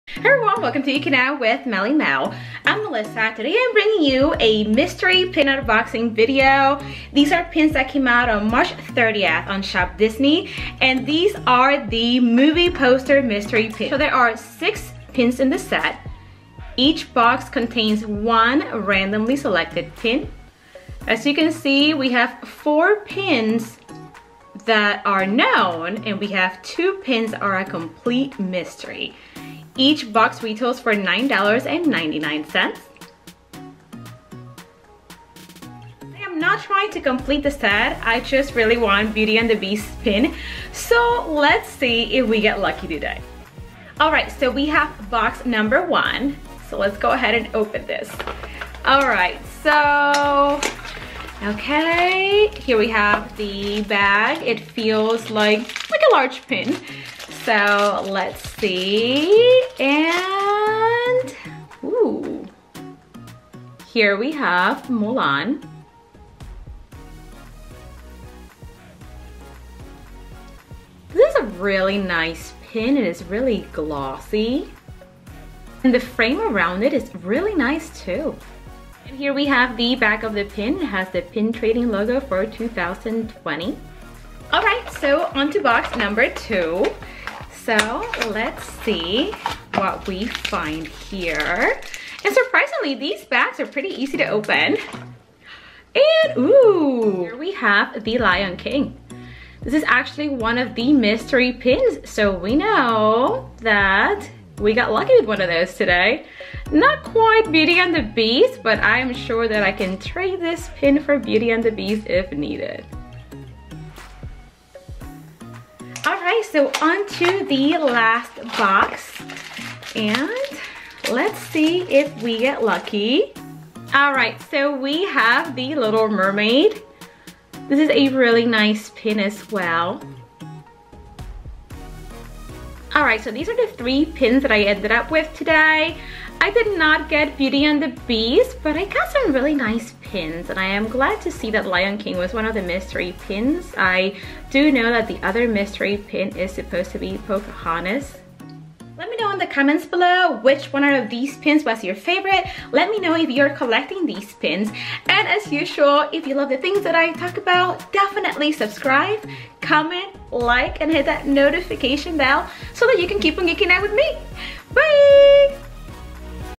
Disney Pin Unboxing | Movie Poster Mystery Pins
On March 30th, ShopDisney released the Movie Poster mystery pins. I ordered 3 boxes and here you’ll see what I got! Did you get these pins too? What did you get? 😉